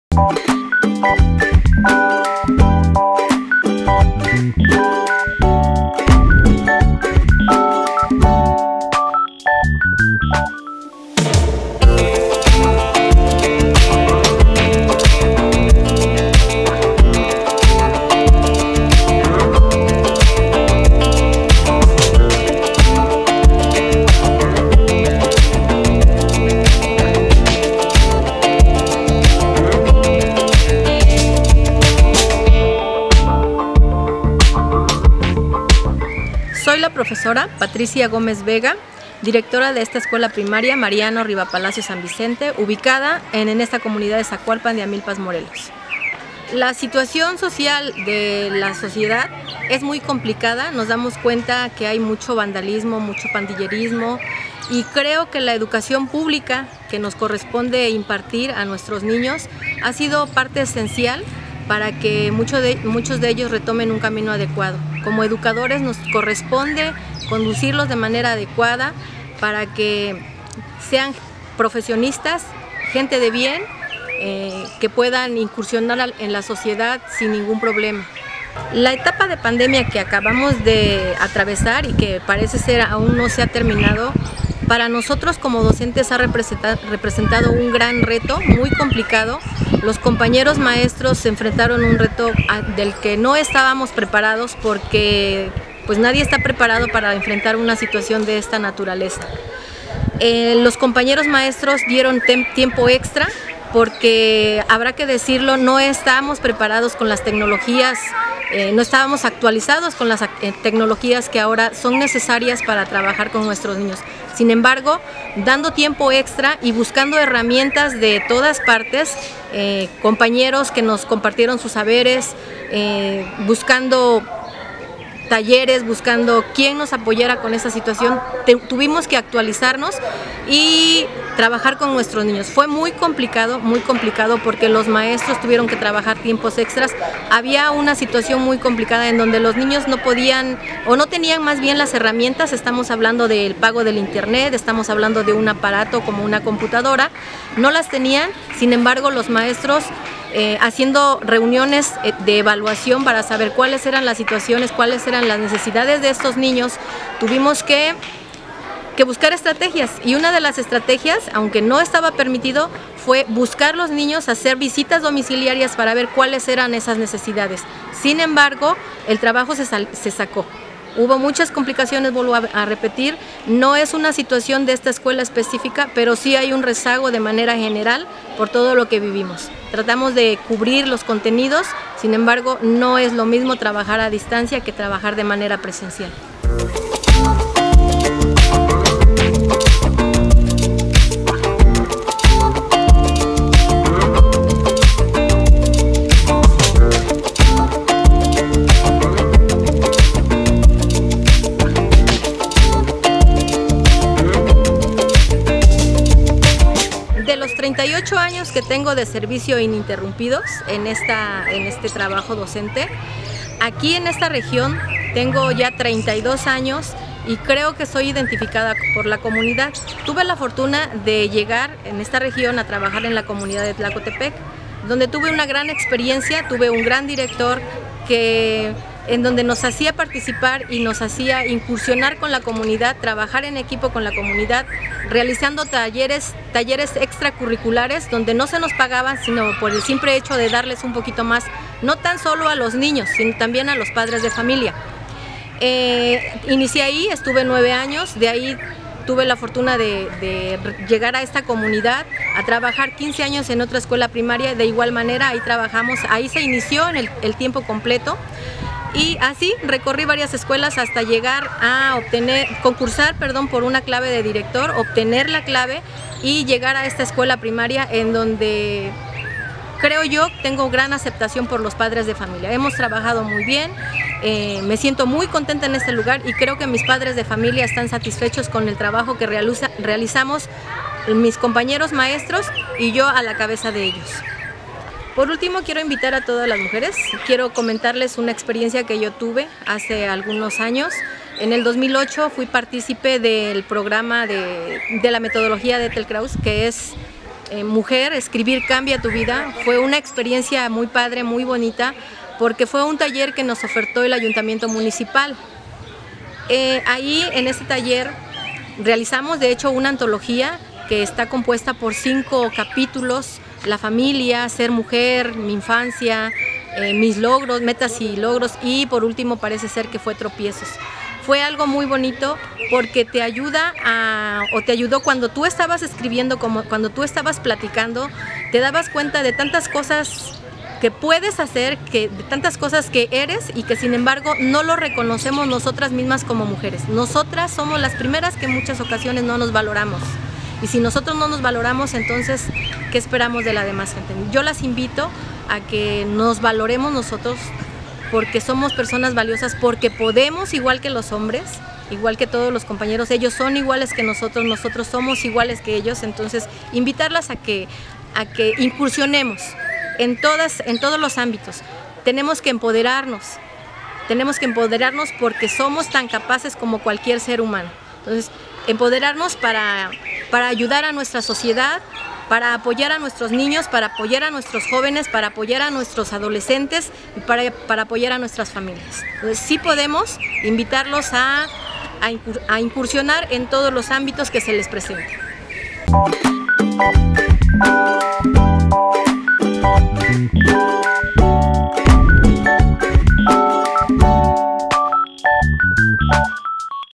Revolucionarias de Hoy - Patricia Gómez Vega | Profesora en Zacualpan de Amilpas
Revolucionarias de hoy: Mujeres sembrando igualdad en el territorio morelense. Patricia Gómez Vega  |  Profesora en Zacualpan de Amilpas. Acceso a la educación para todas, plantear otros modelos educativos con perspectiva de Derechos Humanos.

FaceBook: Instituto de la Mujer para el Estado de Morelos